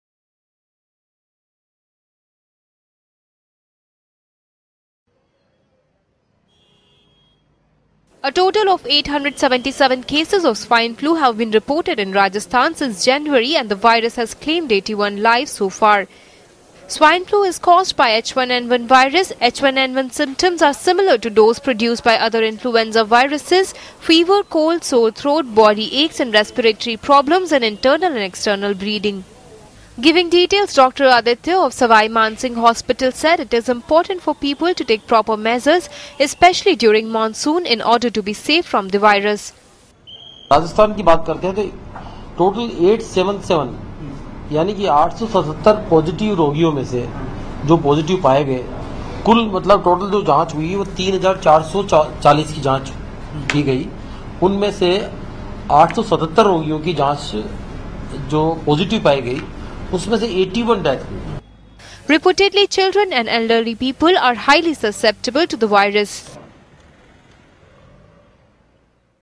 Swine flu claims 81 lives in Rajasthan
A total of 877 cases of swine flu have been reported in Rajasthan since January and the virus has claimed 81 lives so far, said officials.